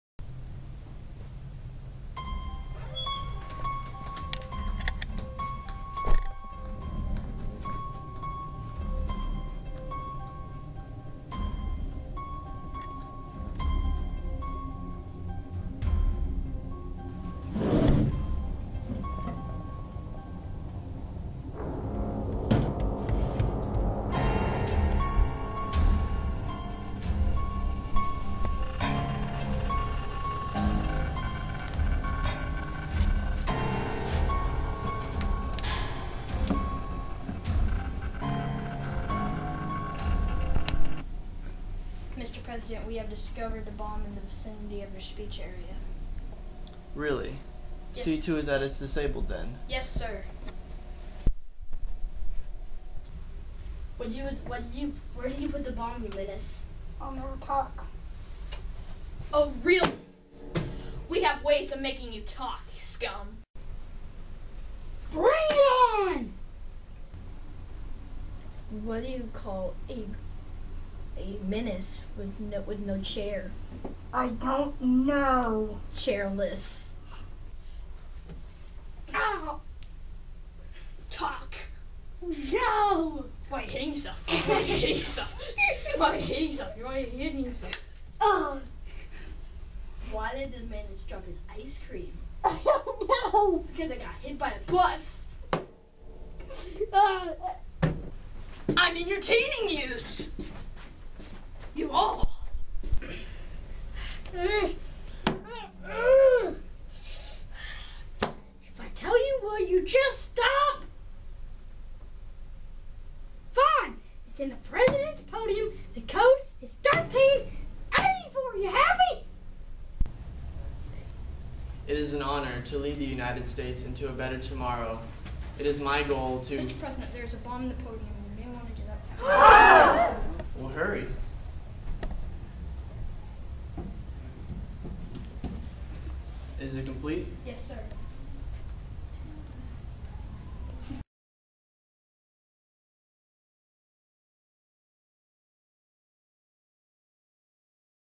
The Villian and the Bomb (2014)
Campers were challenged to come up with a video that utilized time in some way.  A 24-esque short video is what they came up with, complete with a menace, the President of the United States, and government agents.